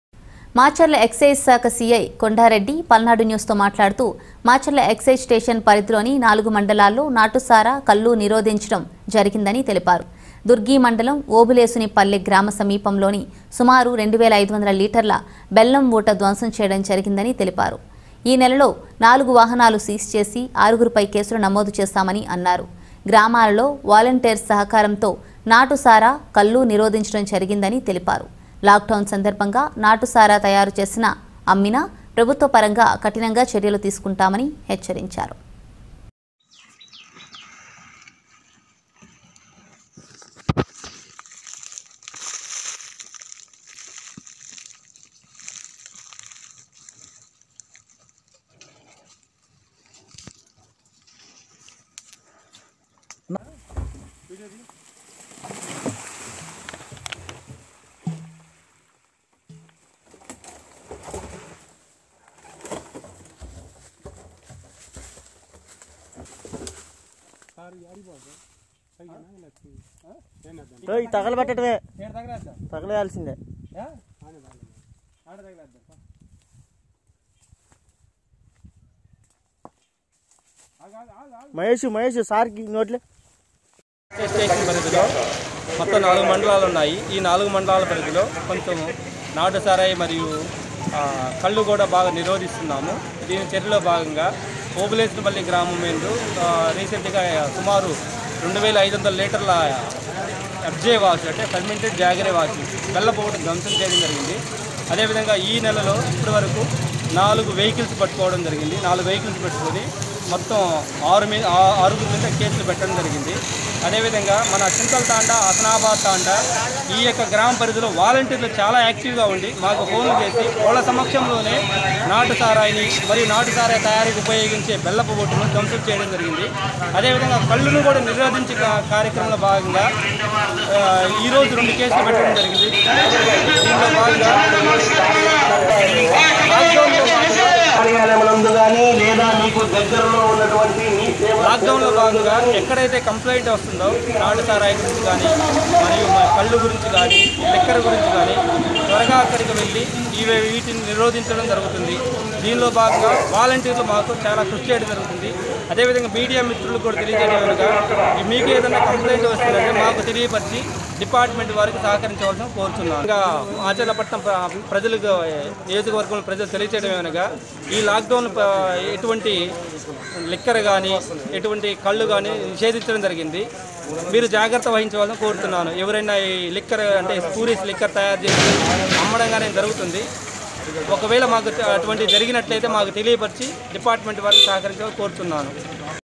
మాచర్ల ఎక్సైజ్ స్టేషన్ పరిధిలోని నాలుగు మండలాల్లో నాటుసారా కల్లు నిరోధించడం
మాచర్ల ఎక్సైజ్ శాఖ సిఐ కొండారెడ్డి పల్నాడు న్యూస్ తో మాట్లాడుతూ మాచర్ల ఎక్సైజ్ స్టేషన్ పరిధిలోని  నాలుగు మండలాల్లో నాటుసారా కల్లు నిరోధించడం జరిగిందని తెలిపారు దుర్గి మండలం ఓబులేసుని పల్లే గ్రామ సమీపంలోని సుమారు 2500 లీటర్ల బెల్లం ఊట ధ్వంసం చేయటం జరిగిందని తెలిపారు ఈ నెలలో నాలుగు వాహనాలు సీజ్ చేసి ఆరుగురి పై కేసులు నమోదు చేశామని అన్నారు గామాలలో వాలెంటైర్స్ సహకారం తో నాటు సారా,కల్లు నిరోధించడం జరిగిందని తెలిపారు లాక్ డౌన్ సందర్భంగా నాటు సారా, తయారు చేసిన అమ్మినా ప్రభుత్వ పరంగా కఠినంగా చర్యలు తీసుకుంటామని హెచ్చరించారు.